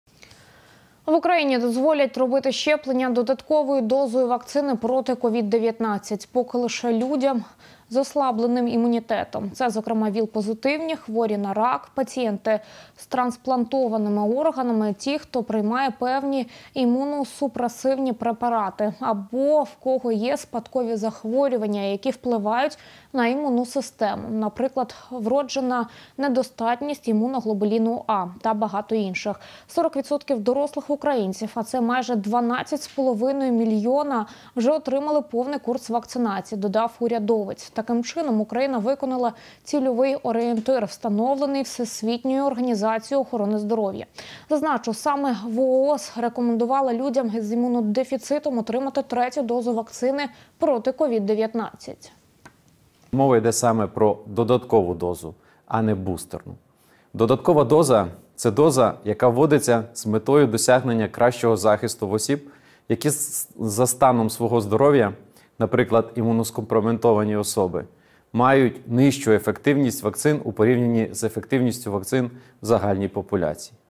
В УКРАИНЕ ПОЗВОЛЯТ ДЕЛАТЬ ДОПОЛНИТЕЛЬНУЮ ПРИВИВКУ ПРОТИВ COVID - 19
В Украине позволят делать прививку дополнительной дозой вакцины против COVID - 19. Пока лишь людям с ослабленным иммунитетом. Это, в частности, ВИЧ-позитивные, больные раком, пациенты с трансплантированными органами, и те кто принимает определеные иммуносупрессивные препараты, или у кого есть наследственные заболевания, которые влияют на иммунную систему(например, врожденная недостаточность иммуноглобулина A) и многие другие. 40 % взрослых украинцев, а это почти 12 млн уже получили полный курс вакцинации, прибавил правительственный чиновник. Таким образом Украина выполнила целевой ориентир, установленный всемирной организацией Охраны Здоровья. Отмечу, именно ВОЗ рекомендовала людям с иммунодефицитом получить третью дозу вакцины против COVID - 19.